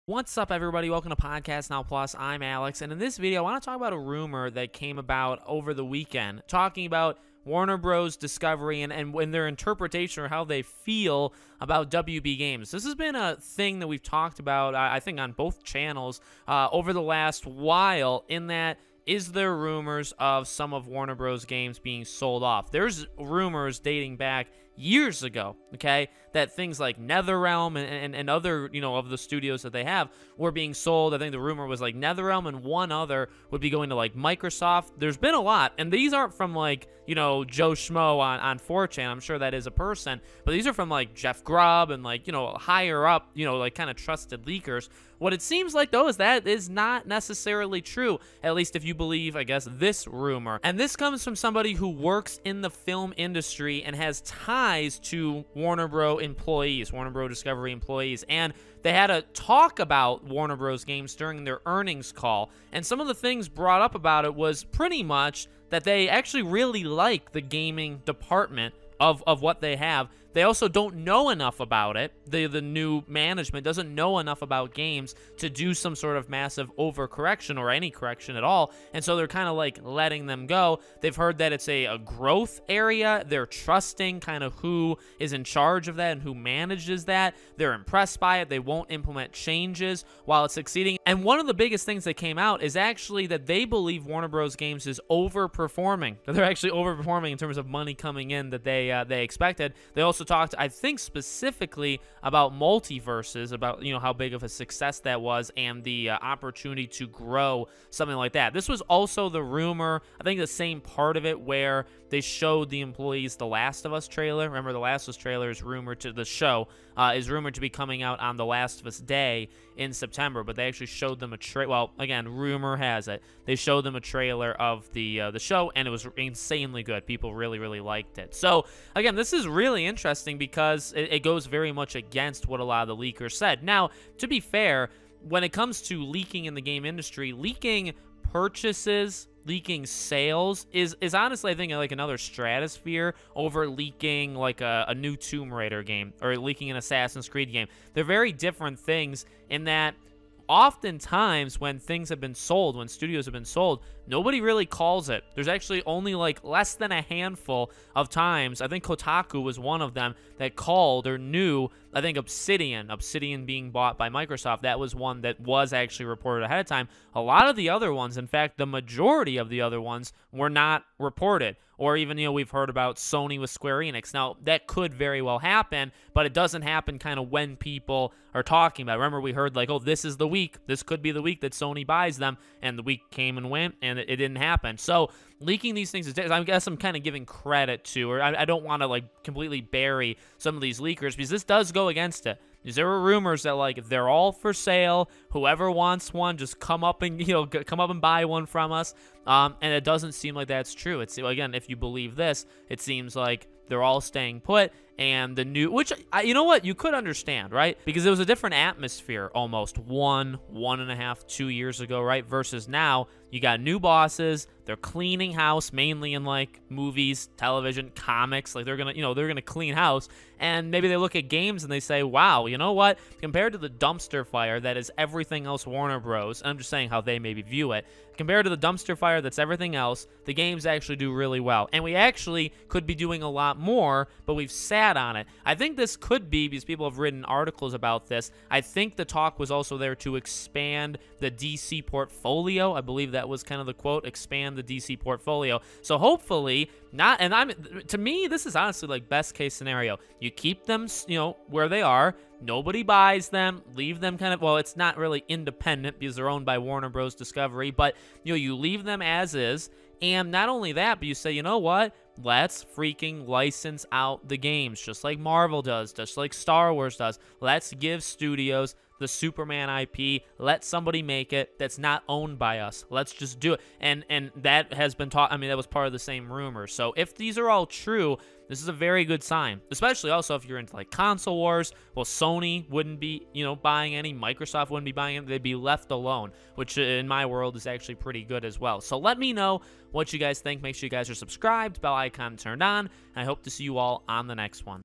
WB Games Not In Danger Of Being Sold - RUMOR
Let's talk about a rumor that WB Games is in no danger of being sold off.